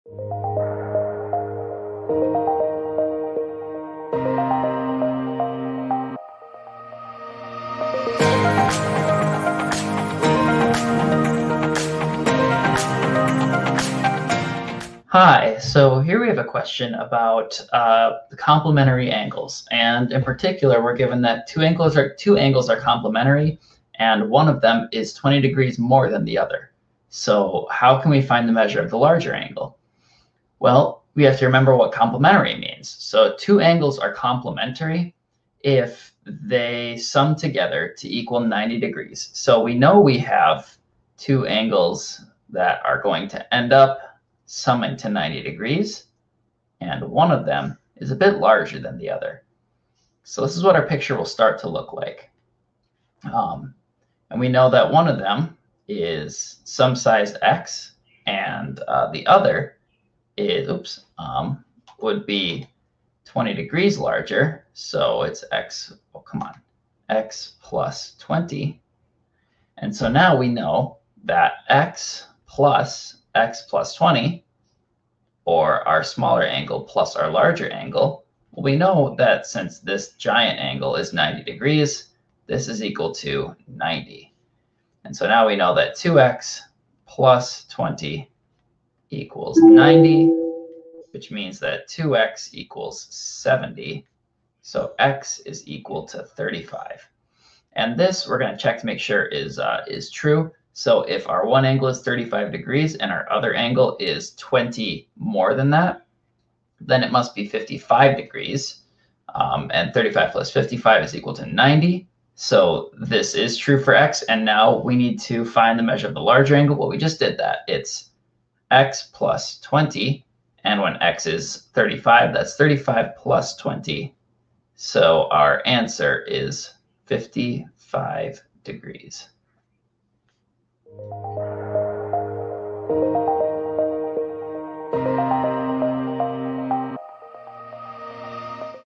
Complementary Angles
Question: two angles are complementary. one angle measures 20 degrees more than the other angle. find the measure of the larger angle.
two angles are complementary. one angle measures 20 degrees more than the other angle. find the measure of the larger angle.

Answered By:

Keith S.
PhD Student & Tutor Specializing in Middle to High School Math